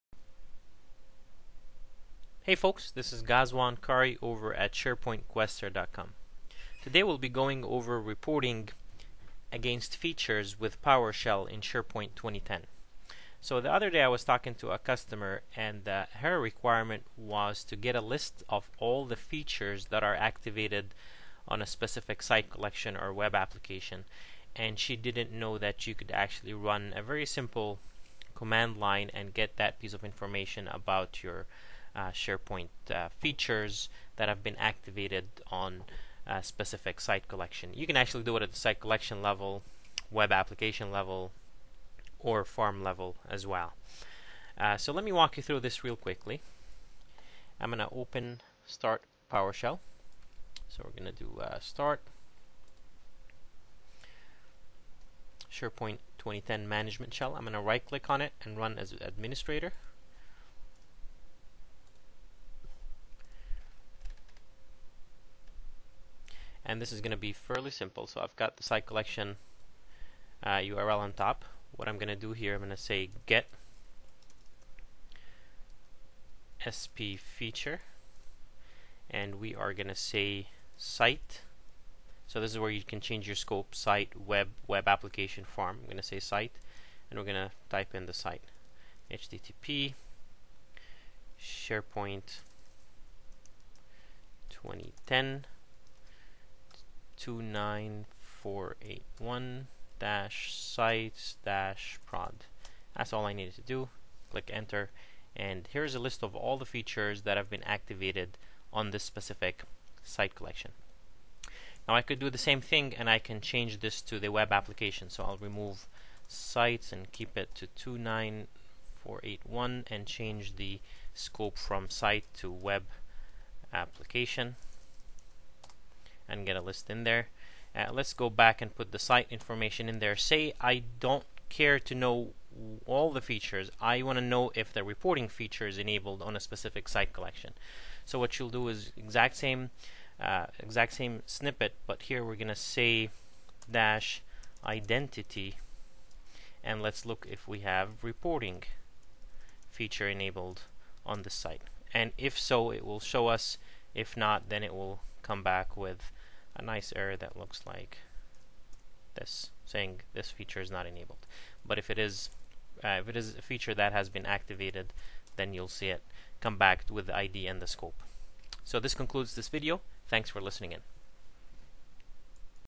Reporting on Activated Site Collection Features in SharePoint 2010 using Windows PowerShell.wmv
This video walks through the process of generating a report on all activated site collection features in SharePoint 2010 using Windows PowerShell. The video also shows how you can report against active features at the web application level as well as reporting on a specific feature to see if it has been activated or not at the site collection level. for more information about this video or to copy the script, please